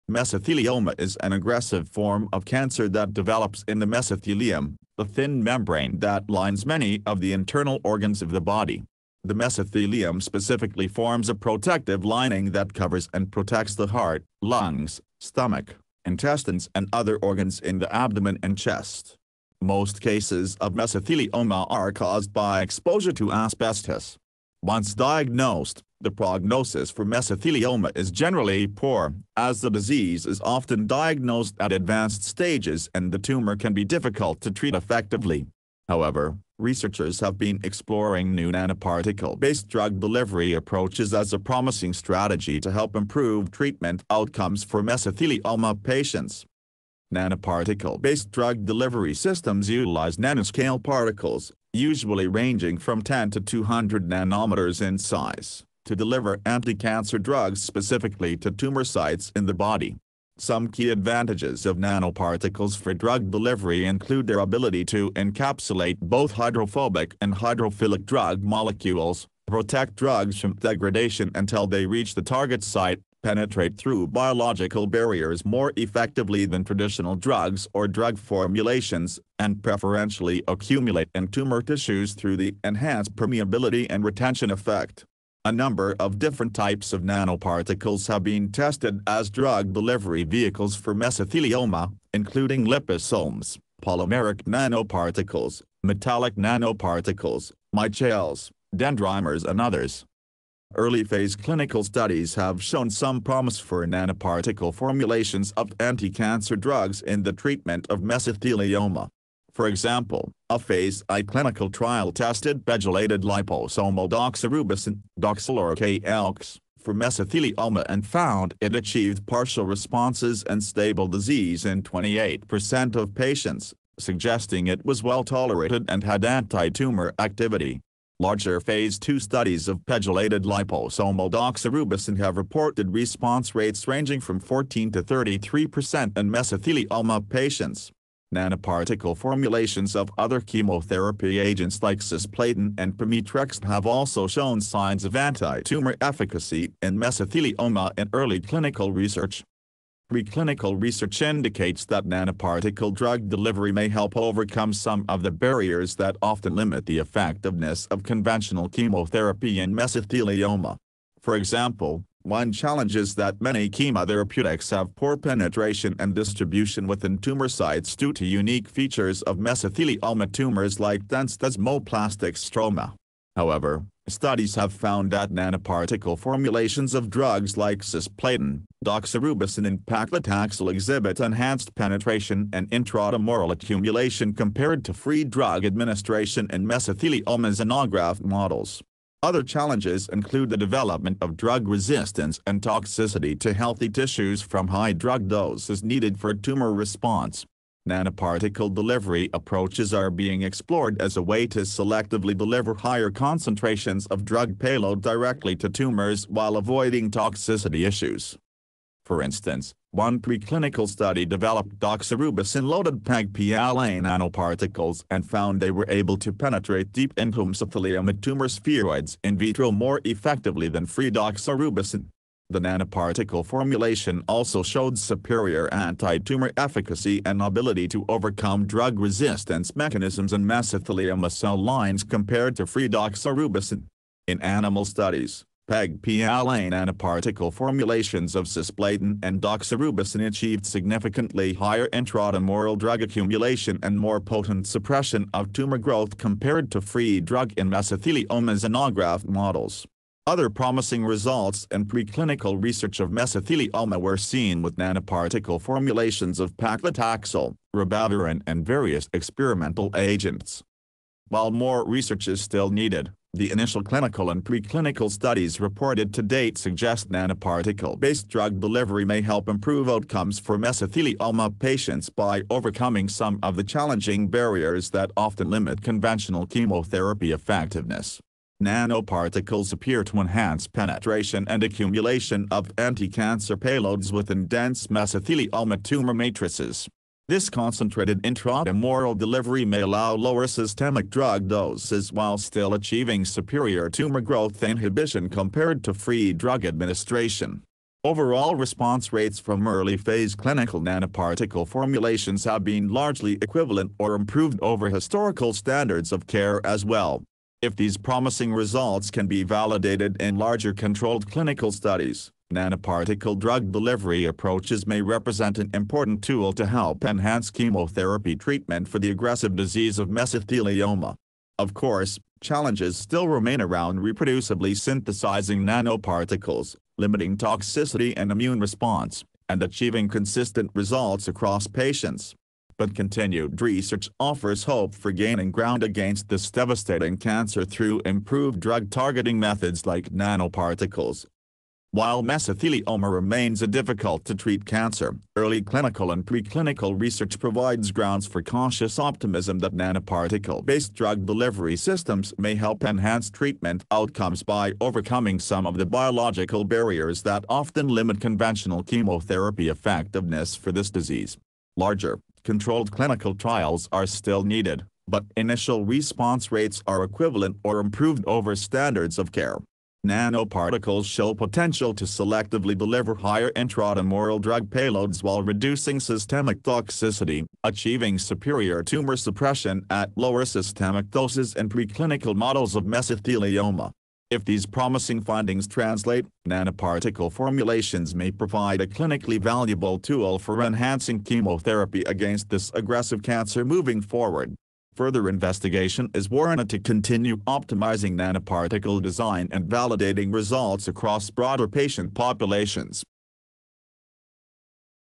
WHAT IS THE SUCCESS RATE OF NANOPARTICLE BASED DRUG DELIVERY IN MESOTHELIOMA?
🔗 The use of nanoparticles as drug delivery vehicles for the treatment of mesothelioma is a promising approach that researchers have been actively exploring in recent years. More research is still needed to definitively determine the overall success rate of this approach. Some key points regarding nanoparticle-based drug delivery and mesothelioma:

Mesothelioma is an aggressive form of cancer that develops from the mesothelium, the protective lining that covers most of the body's internal organs. It is caused by exposure to asbestos and is considered incurable once diagnosed due to chemoresistant tumors and the inability of drugs to reach therapeutically effective concentrations in diseased tissues. Nanoparticles that are capable of encapsulating and delivering high drug payloads specifically to tumor sites may help overcome this challenge.

A number of preclinical studies have shown that nanoparticles can successfully deliver various chemotherapy drugs, cytokines, and other compounds to mesothelioma tumors in animal models. For example, nanoparticles carrying doxorubicin, paclitaxel, pemetrexed, or other drugs have exhibited improved antitumor efficacy compared to free drug administration alone. Enhanced permeation and retention effects allow nanoparticles to accumulate in the tumors.

Translating these preclinical findings to effective clinical results in human patients has proven more difficult to achieve. While some early phase clinical trials of nanoparticle formulations for mesothelioma have demonstrated favorable safety profiles and signs of anti-tumor activity, successful treatment of late-stage disease remains elusive. Barriers include the inability to achieve uniform particle distribution throughout tumors and the body's natural defenses against foreign nanoparticles.

Larger phase 3 trials are still lacking, and more research is needed to better optimize the design of nanoparticles for target delivery to mesothelioma tumors in patients. Key factors that could potentially improve upon preclinical success rates in people include the use of active targeting moieties, controlled intracellular drug release properties, biocompatible surfaces, and combination therapy approaches that leverage the benefits of nanoparticles alongside other modalities.

While preclinical data shows promise, the actual success rate of complete response or long-term remission for patients with mesothelioma treated exclusively with nanoparticle drug delivery systems remains unknown at this time, as clinical translation remains a work in progress. Continued innovation in the design and application of engineered nanomedicine platforms offers hope that the barriers preventing widespread effectiveness in human mesothelioma patients can be overcome in the future. More definitive clinical trials are still required to determine the realistic success potential of this approach.